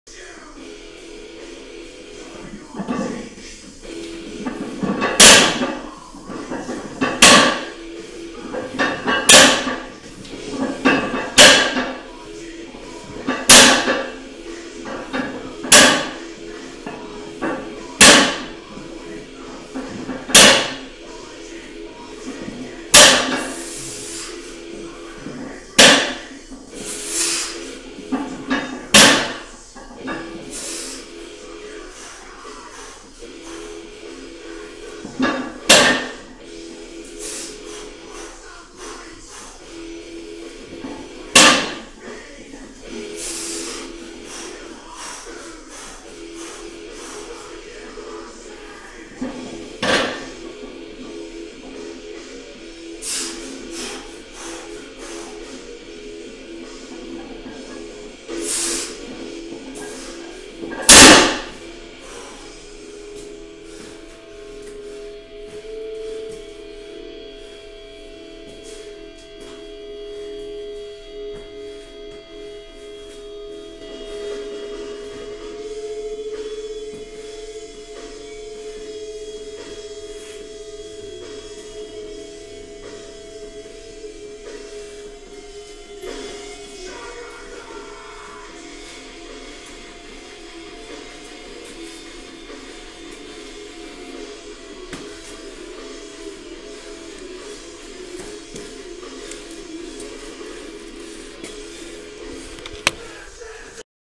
Deadlift 15*x495 touch and go
I know no one else is going to count that 15th rep, but since it's a training lift I get to do it.  Especially if you get points for effort, because that moonwalk at the end was totally unscripted.  This is a lifetime PR, to say nothing of a post ACL reconstruction PR.